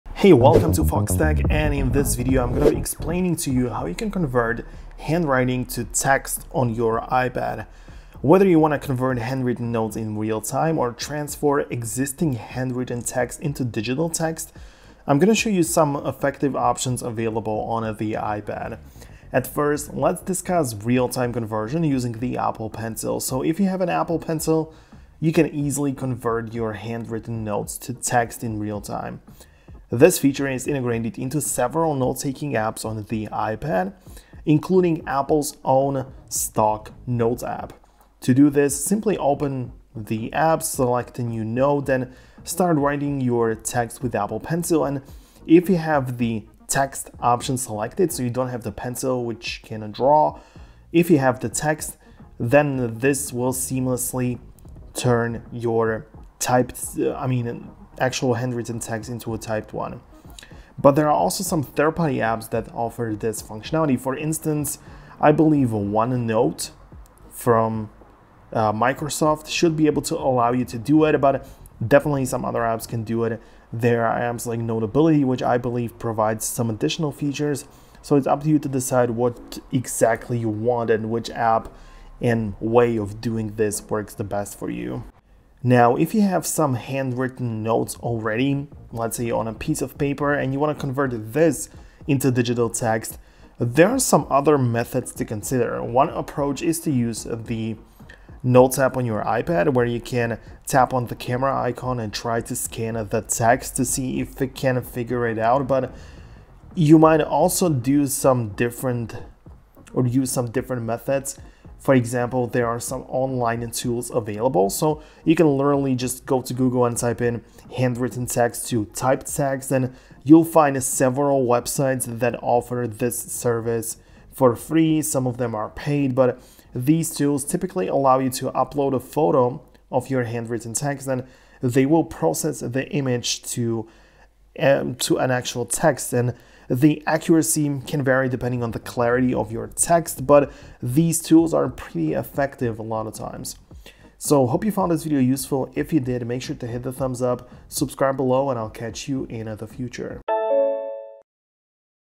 Can You Convert Handwriting to Text on iPad (tutorial)
In this video, we explore whether you can convert handwriting to text on your iPad. Learn about the tools and apps available to transform your handwritten notes into editable text. Follow our guide to see how easy it is to digitize your handwriting for better organization and productivity. Watch now to discover the full potential of your iPad for note-taking and text conversion.